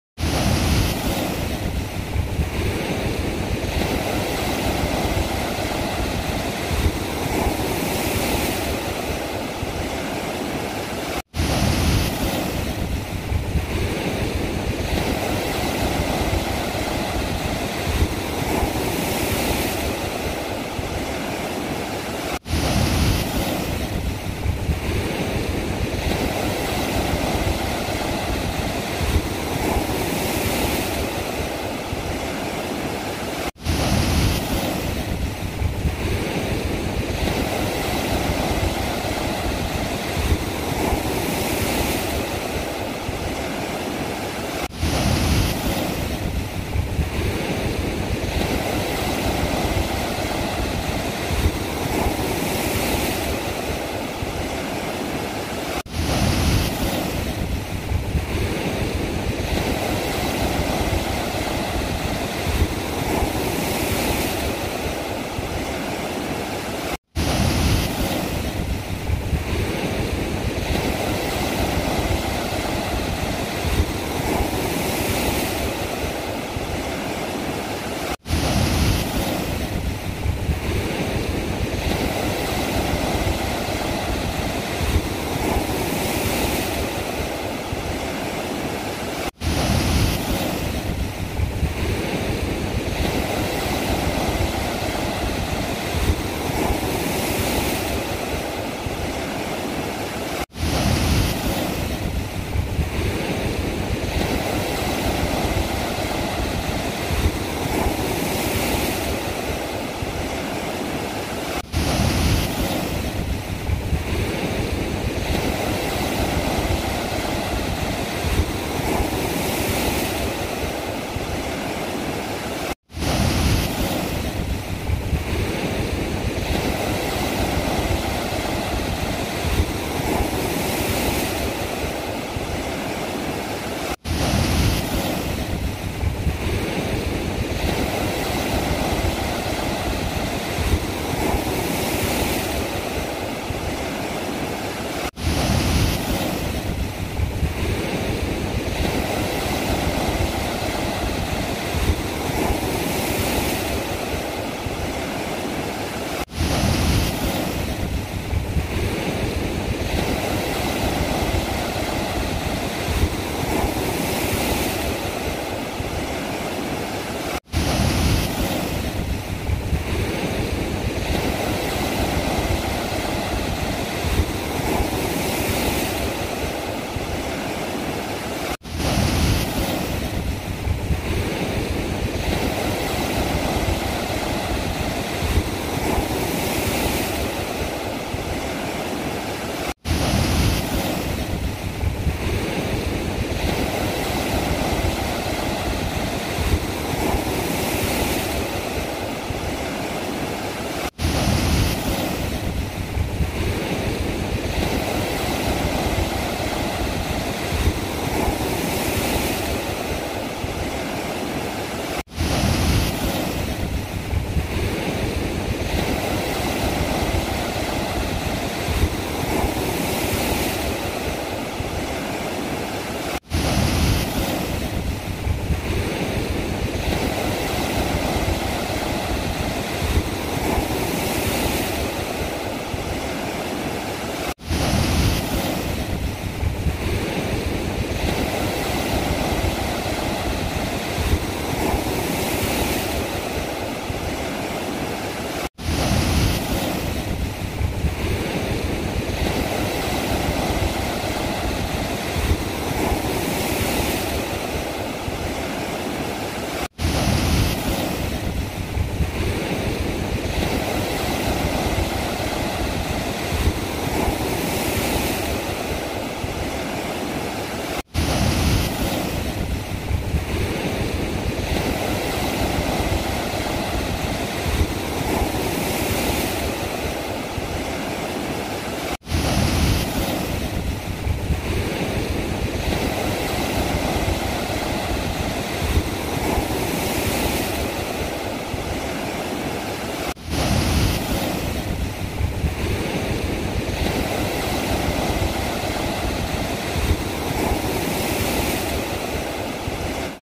5 minutes of Relaxation Waves
Went to a beach and found waves